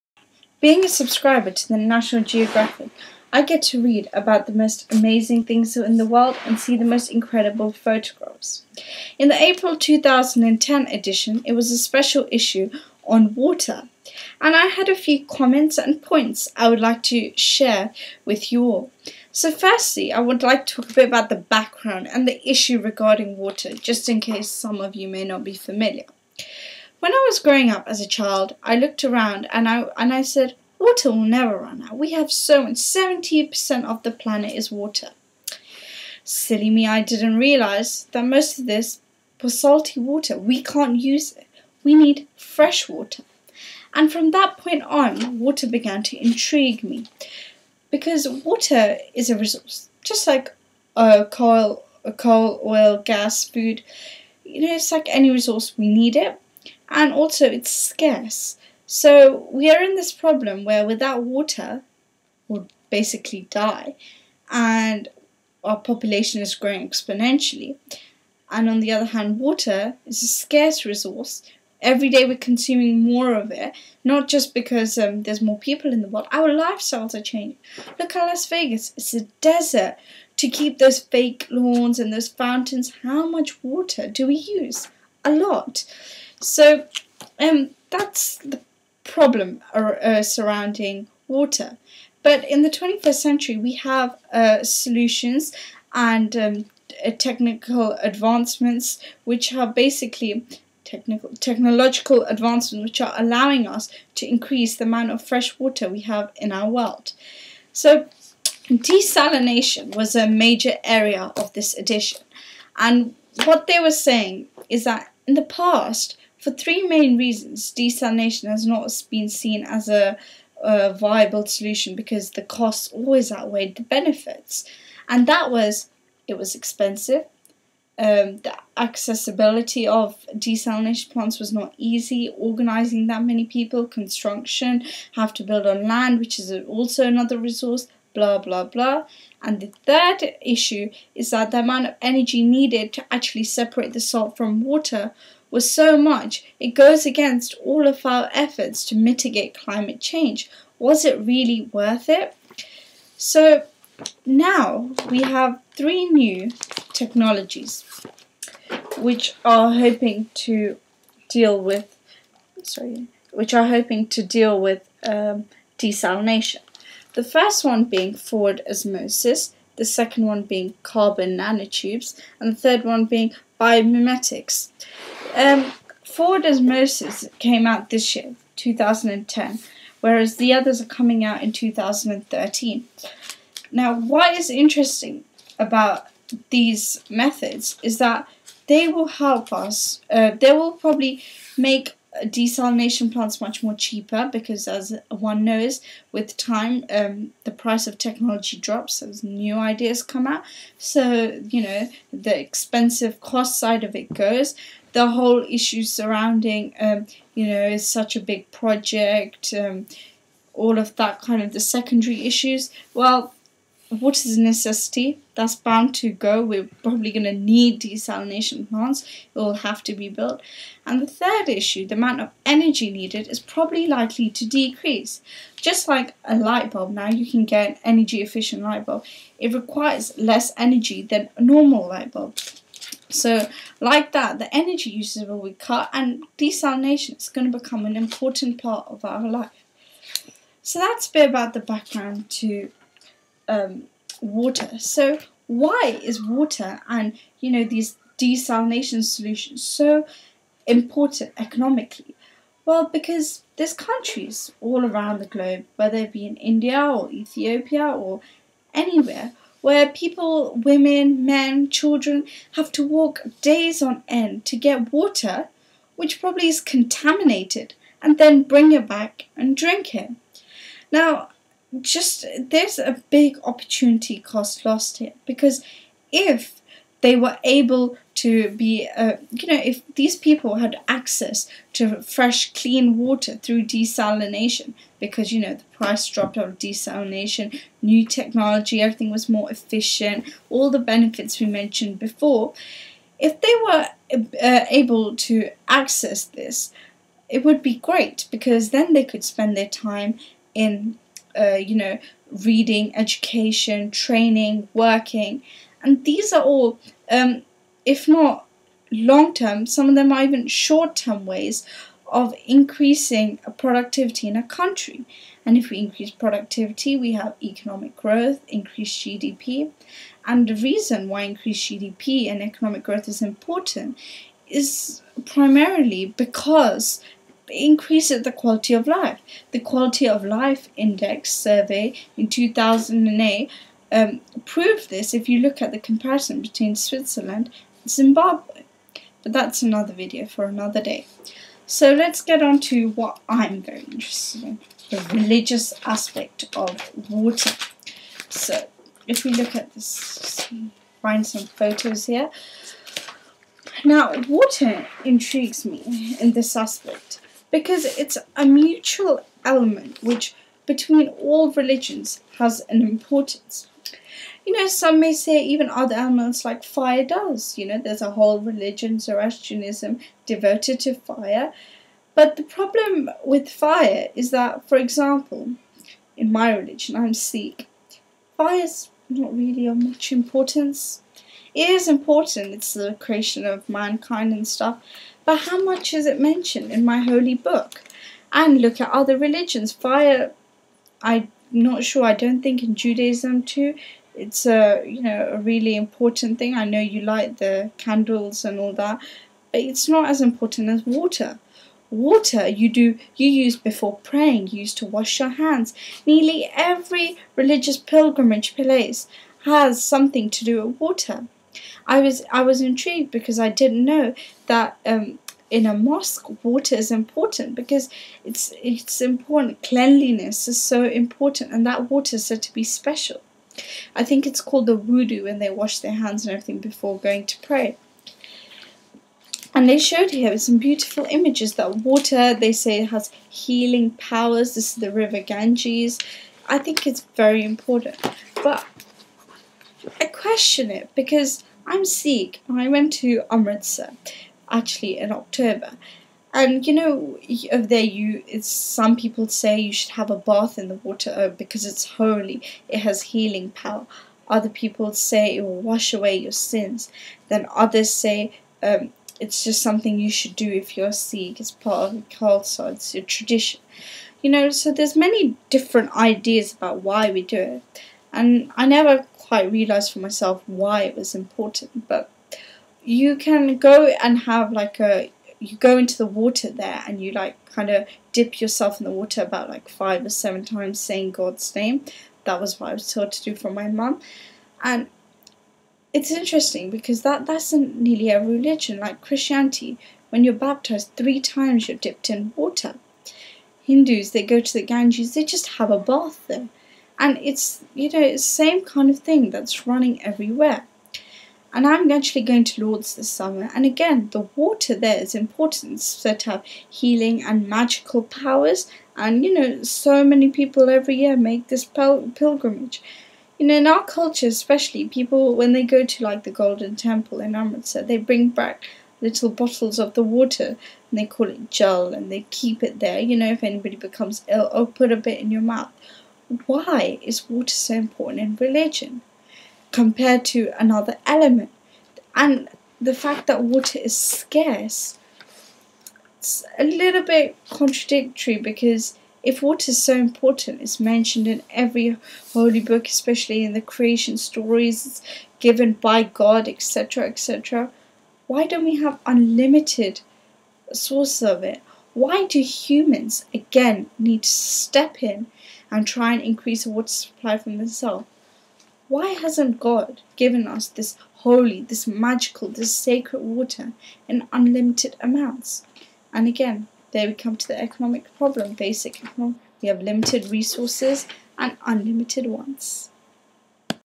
National Geography - Water Issue : My thoughts
In this video, I discuss a few points I had while reading the April 2010 special edition of water issue. I discuss the water crisis and the solution technological advancements in desalination present. Then I goon to discuss what economic benefits this solution can bring, in theory. Finally, I question why out of all elements water is the most important one.